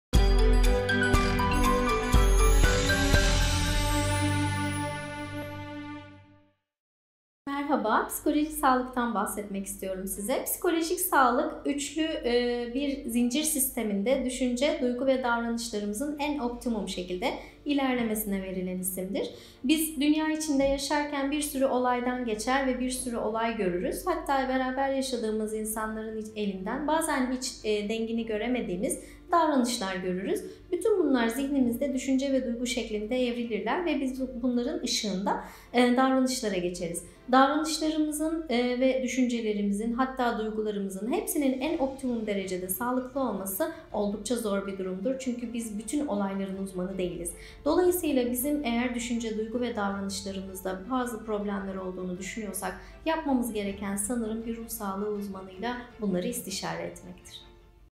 Psikolojik Sağlık Nedir? - Psk. Nergis Öksüz
Psikolojik Sağlık üçlü zincir sisteminde, düşünce, duygu ve davranışlarımızın en optimum şekilde ilerlemesine verilen isimdir.

Bu üçlü zincirin optimum olması kolay değildir, çünkü bizler tüm olayların uzmanı değiliz.